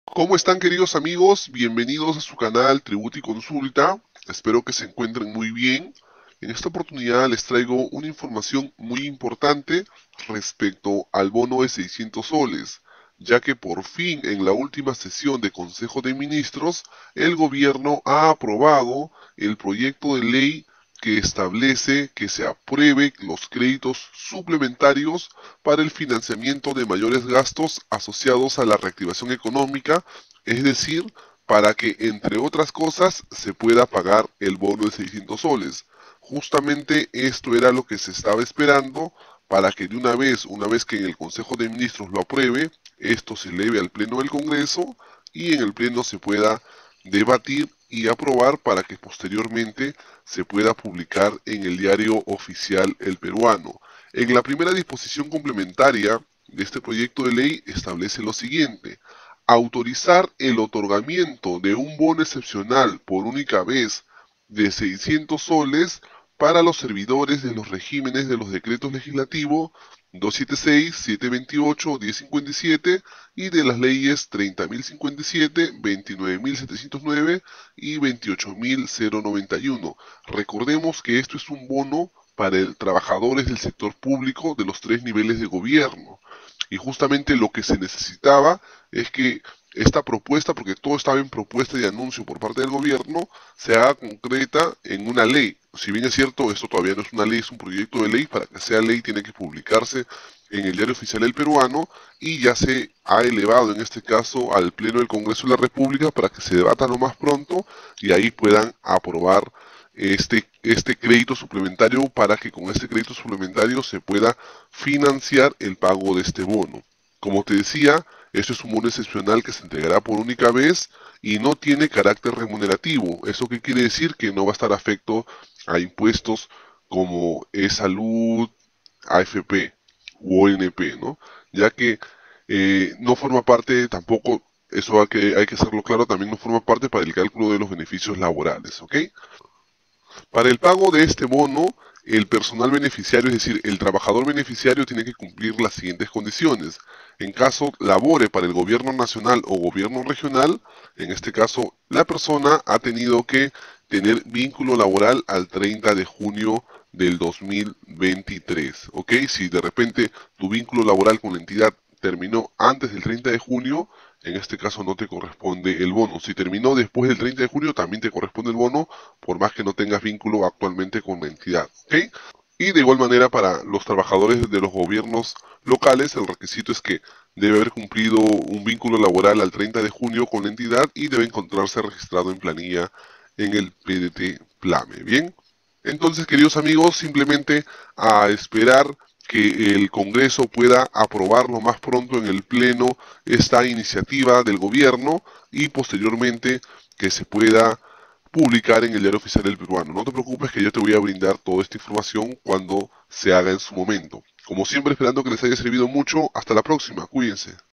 !Buenas noticias! BONO 600 SOLES |Gobierno aprobó P.L. para el inicio del pago del Bono de 600 soles
Bono 600 soles 2023.
Bono para trabajadores del Sector Público.

Gobierno aprobó proyecto de Ley que aprueba el crédito suplementario para el pago del Bono de 600 soles.